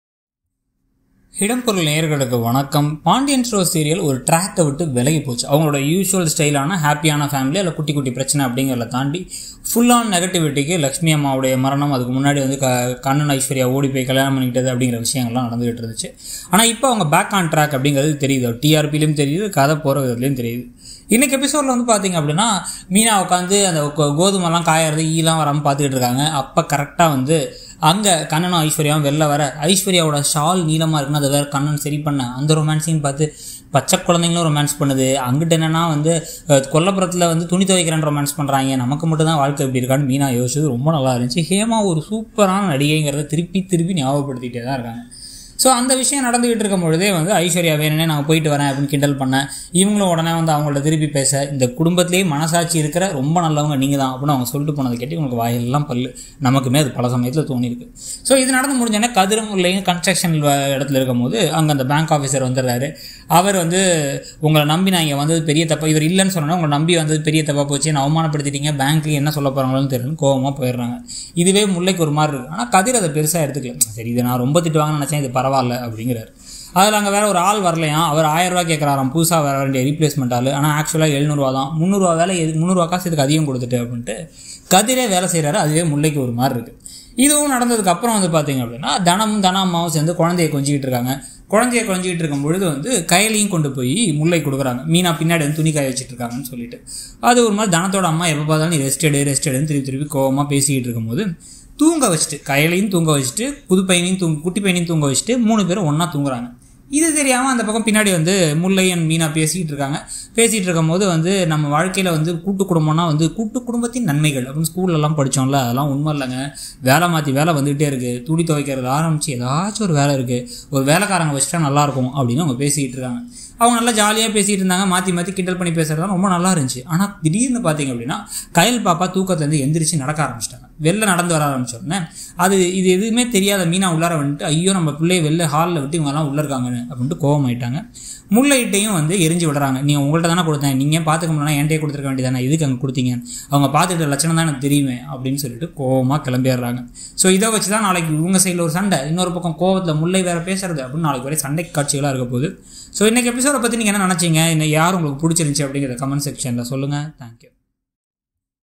Pandian Stores promo shock twist | 28.10.2021 today episode review | Vijay Television
Pandian Stores today episode promo | 28/10/2021 today episode review | prediction | Vijay Television | Meena upset over Dhanam's behaviour and Mullai angry on Moorthy. Mullai blasts the family for Kathir as his idea for ATM is rejected unfairly and Meena fights with Dhanam over Kayal. New baby's naming function celebrations begin & Kannan isn't invited? | Pandian Stores promo 27 October  to 30 October 2021 next week promo